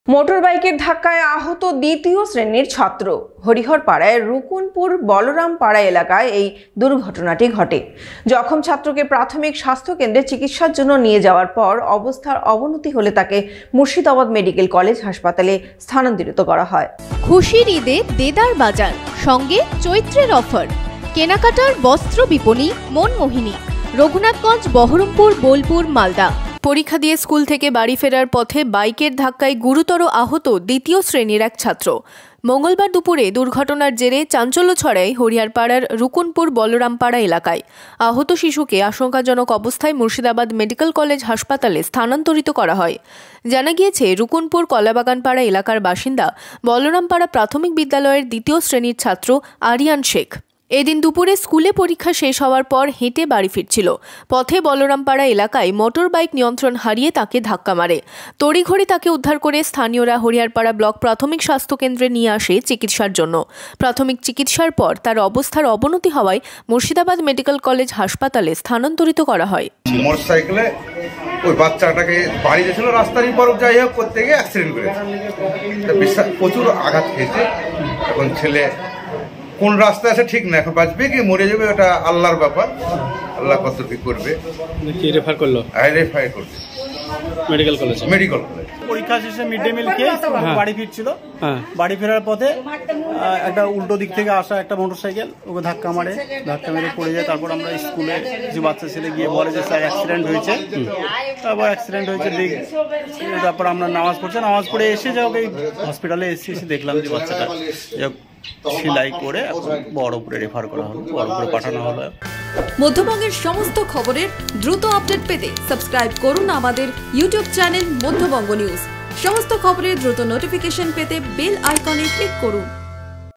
Hariharpara Accident News: হরিহরপাড়ায় বাইকের ধাক্কায় গুরুতর জখম দ্বিতীয় শ্রেণীর ছাত্র
বাইকের ধাক্কায় গুরুতর জখম দ্বিতীয় শ্রেণীর ছাত্র

Madhyabanga News